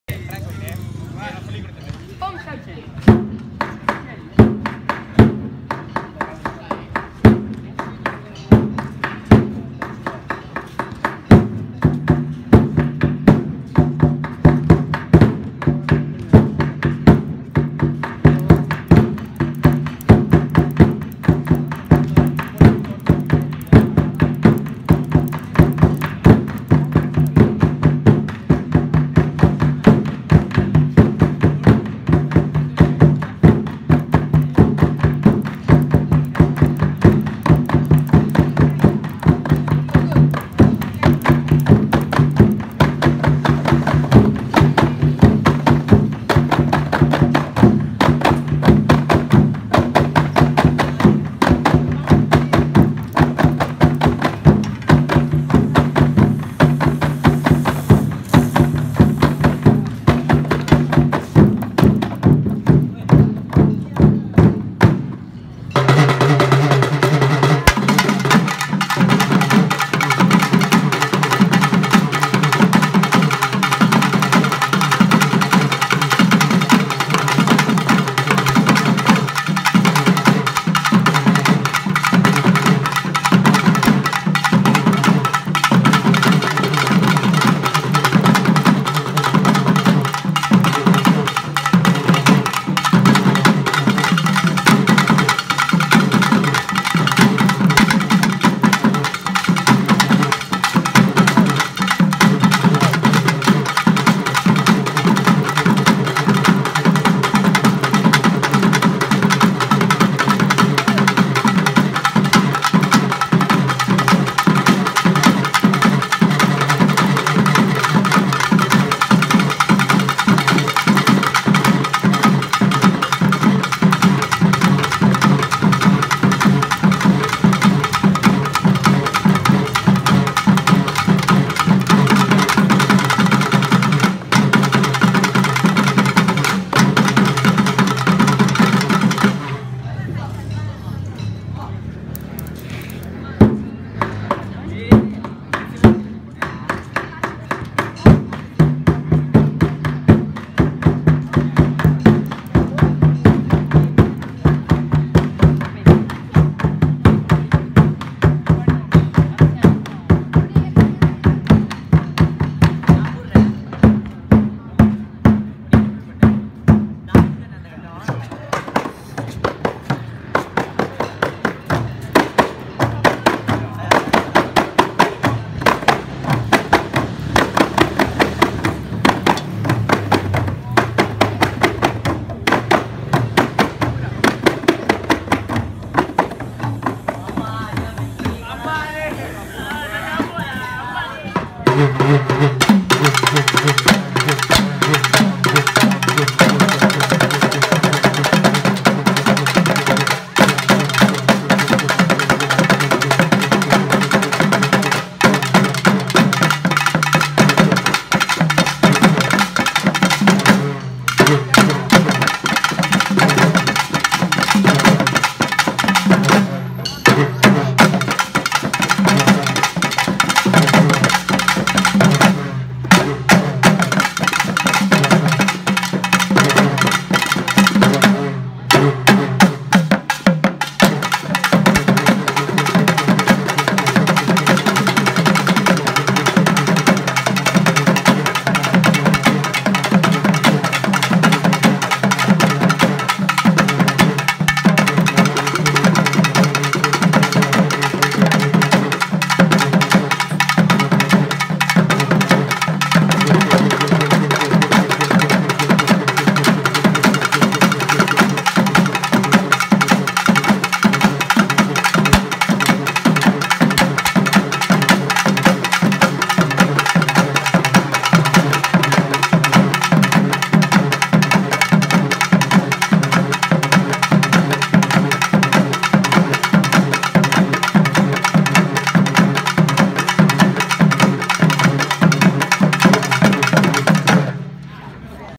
URUMI MELAM KOMPANG 24 SEASONS DRUM (REMIX)
MALAYSIA SCHOOL (SMK Seri Kembangan) students preparing for performance URUMI MELAM KOMPANG 24 SEASONS DRUM (REMIX)